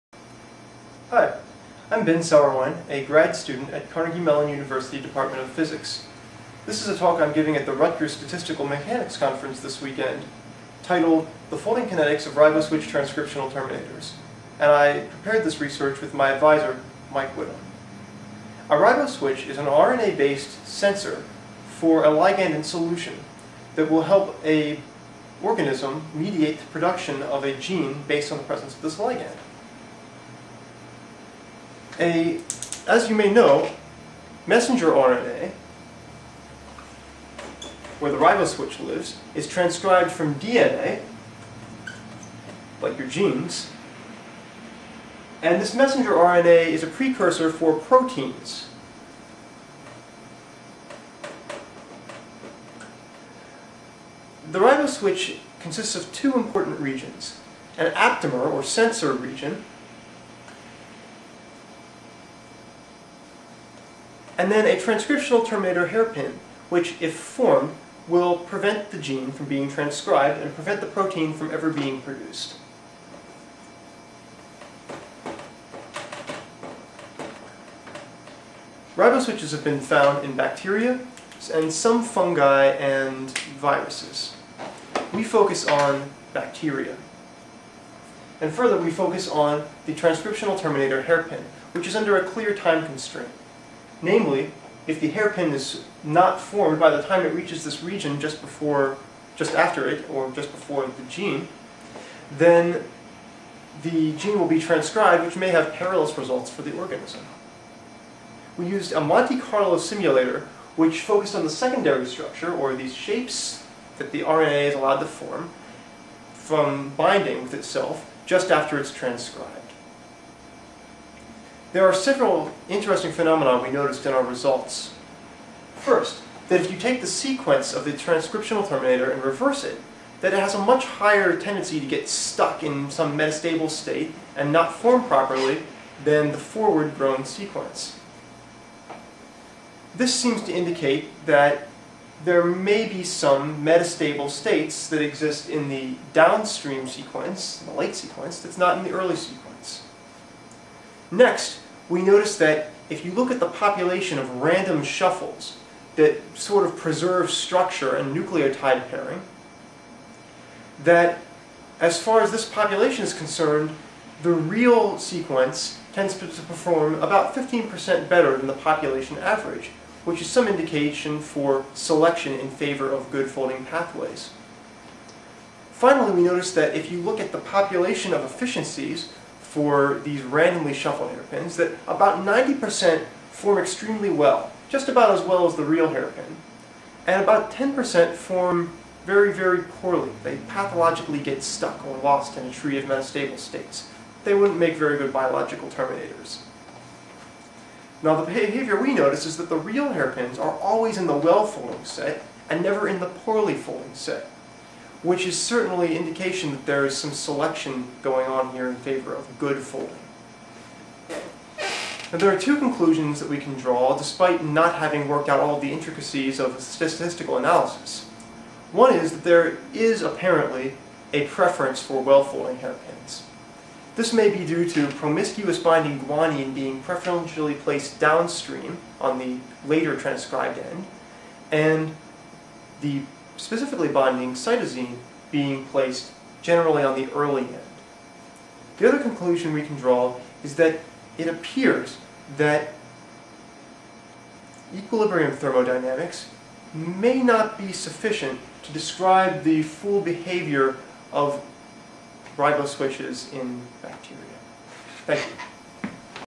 Folding of Riboswitches Short Talk
The YouTube version of my talk for the 2008 Rutgers Statistical Mechanics Conference.

It's only three minutes, and comparatively non-technical.